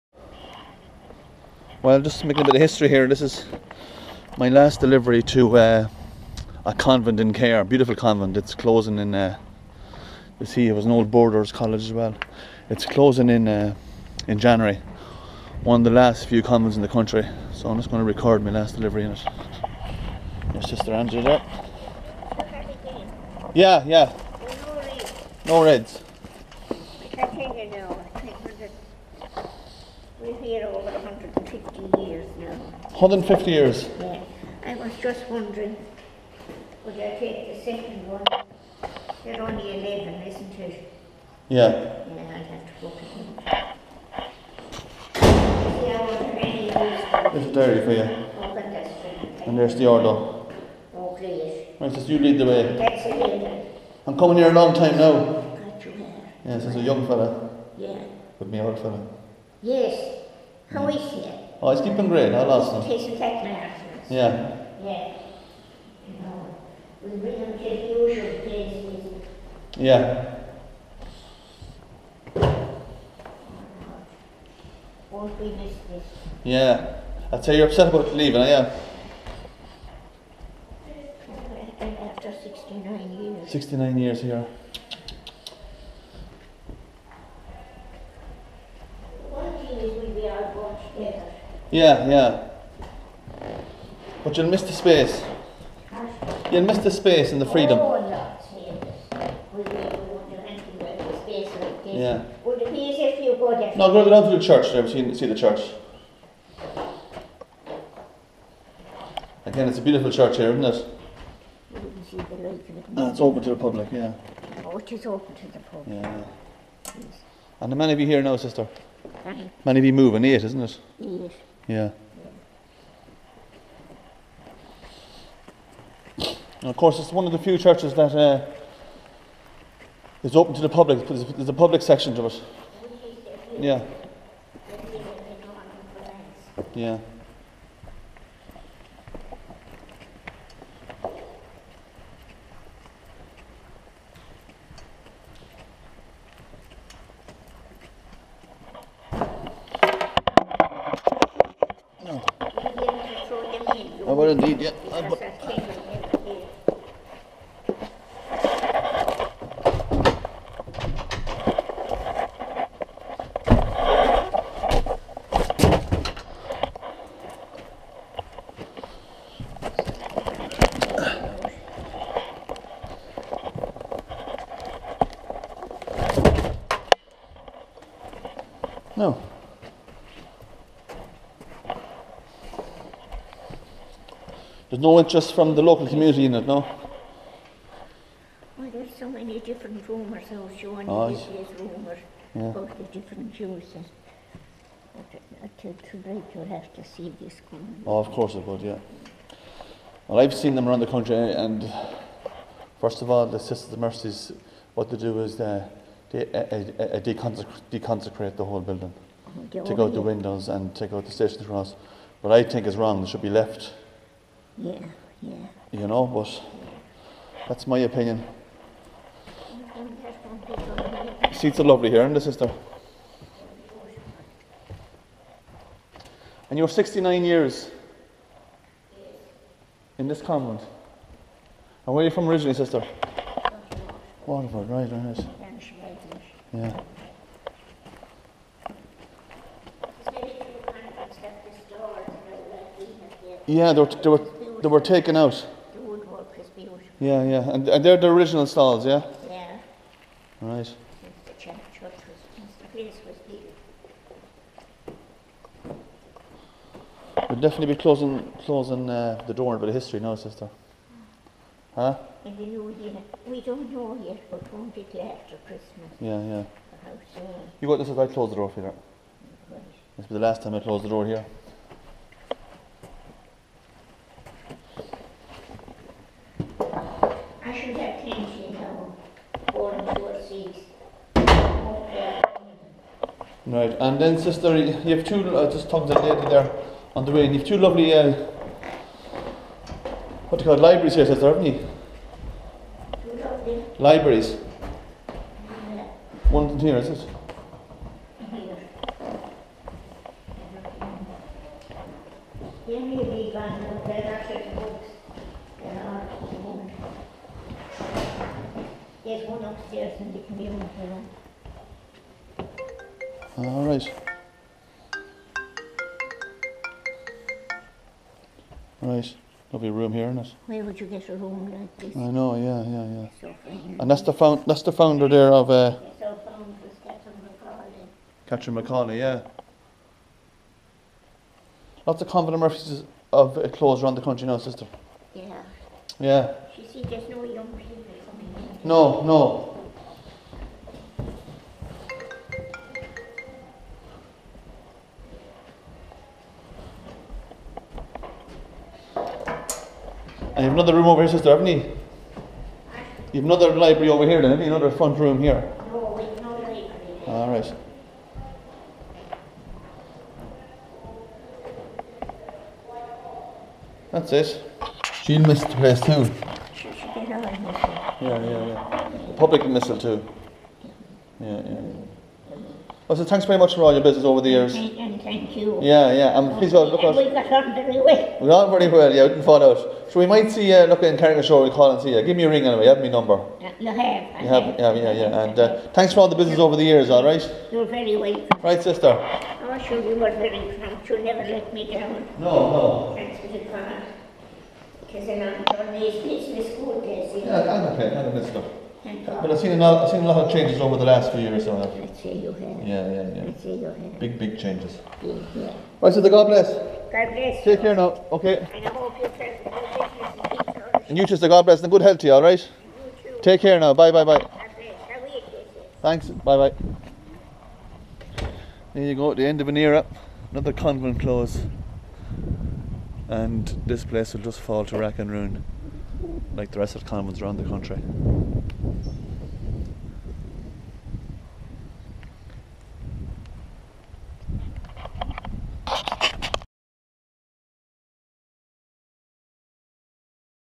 The Last Delivery
MY LAST DELIVERY TO  CAHIR CONVENT OF MERCY ,HAVE BEING DOING BUSINESS HERE FOR THE LASST 35  YEARS ALONG WITH MY FATHER .IT WILL BE MISSED AND I'LL MISS SISTER ANGELA/